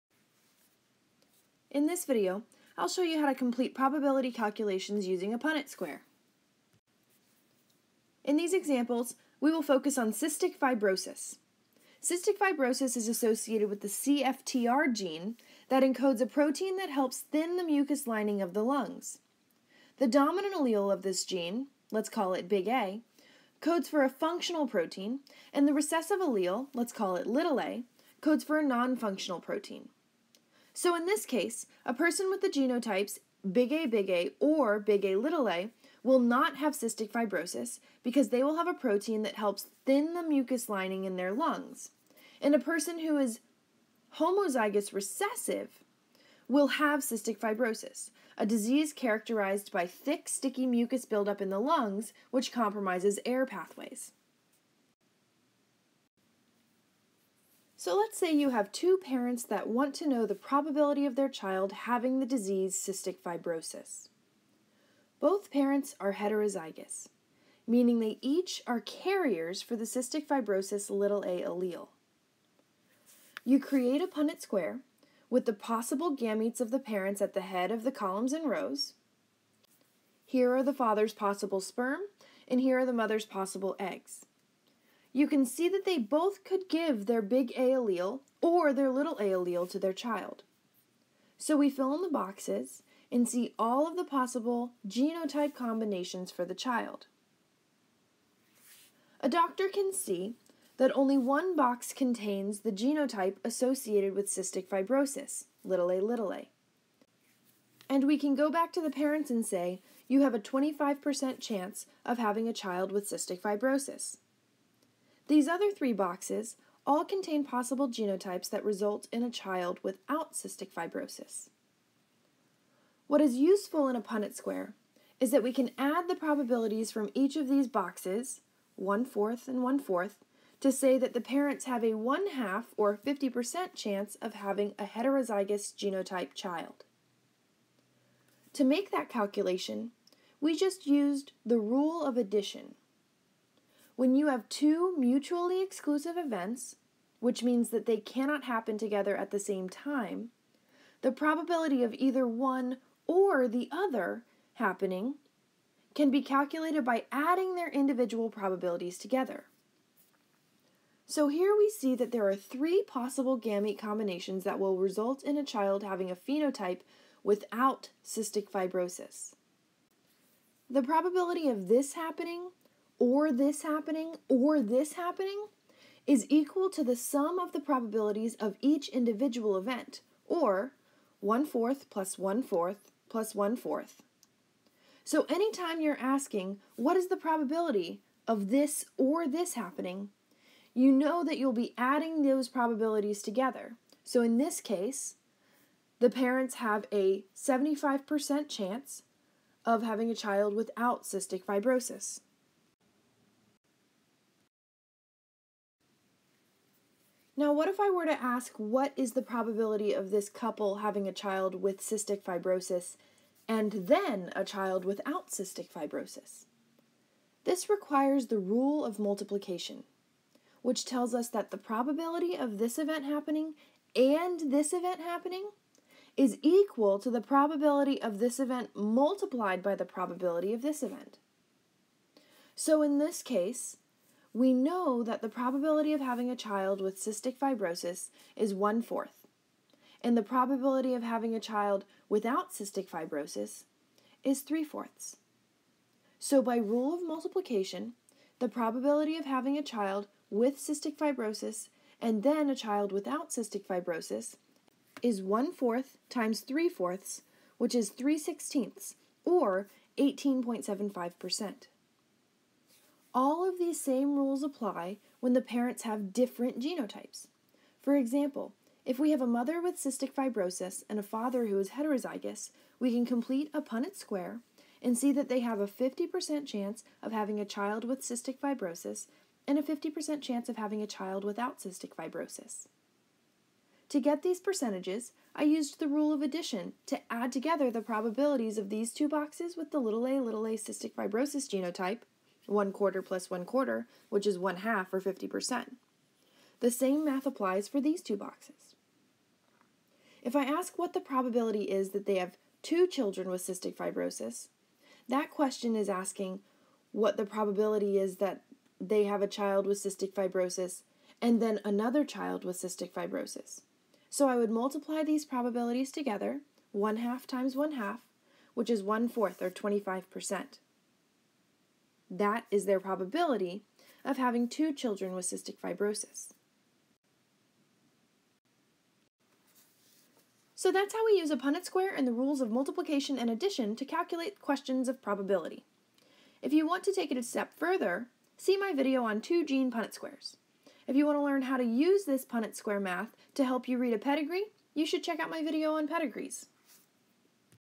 Probabilities and Punnett Squares
How to use the rules of multiplication and addition to solve questions related to possible offspring genotypes.

Taught through the lens of cystic fibrosis.